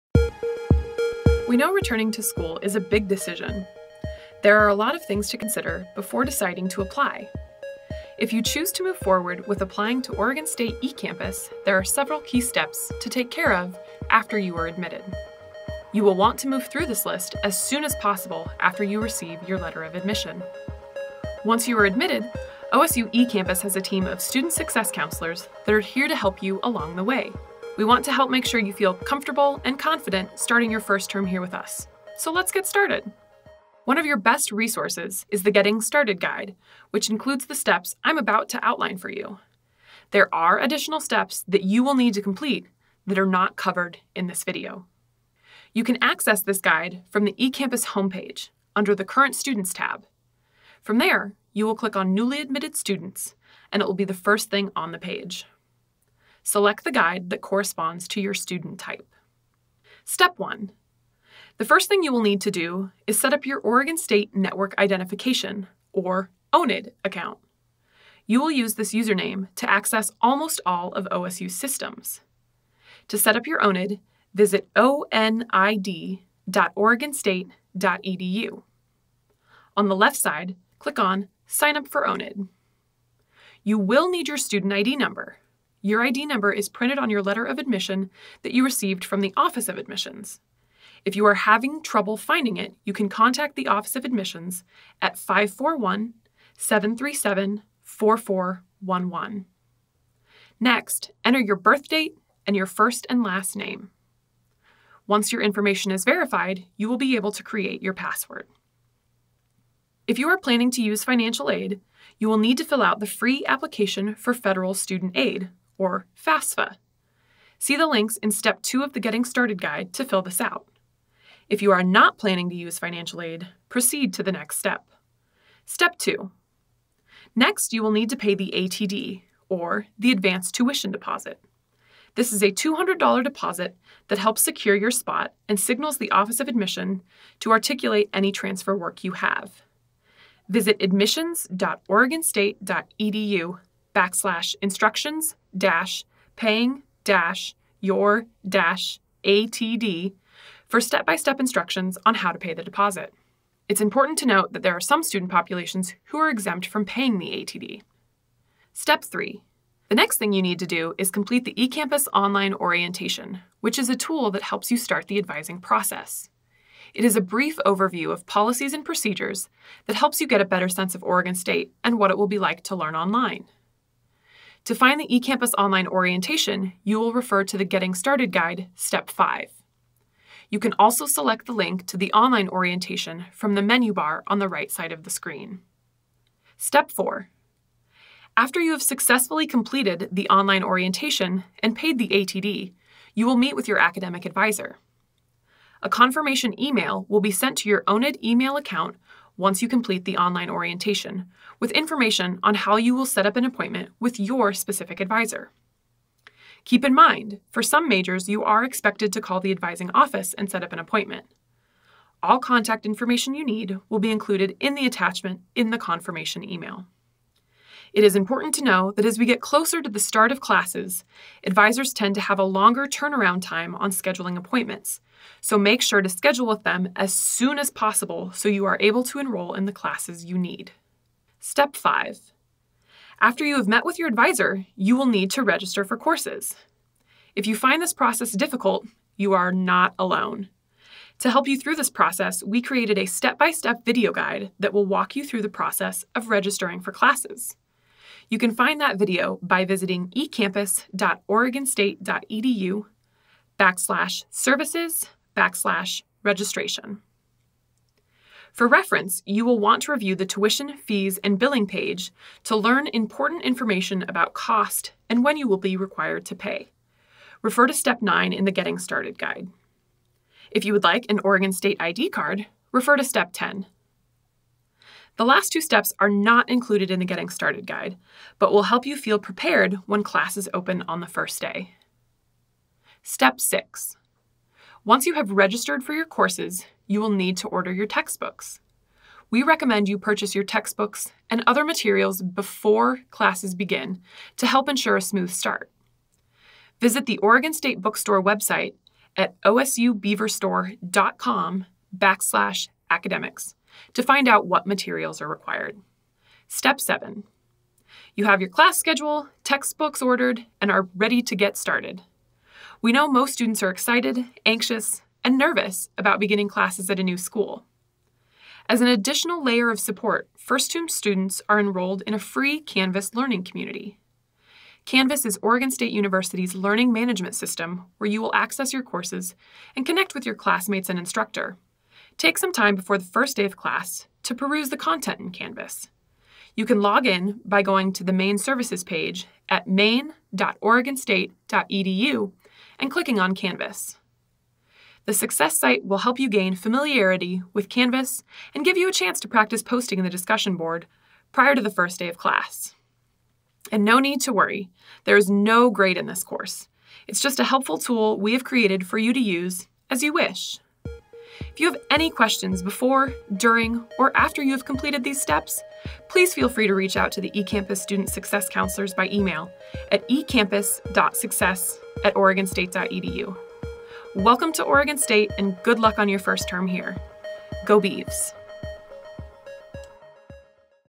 Getting started at OSU Ecampus
We know returning to school is a big decision. There are a lot of things to consider before deciding to apply. If you choose to move forward with applying to Oregon State Ecampus, there are several key steps to take care of after you are admitted. You will want to move through this list below as soon as possible after you receive your letter of admission.